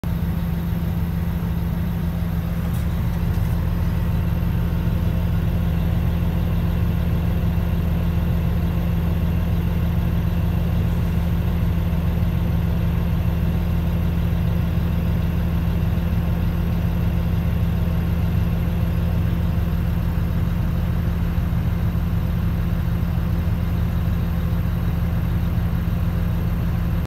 Deere 3154 & Cat D7R building fire guard
Cleaning up a right of way.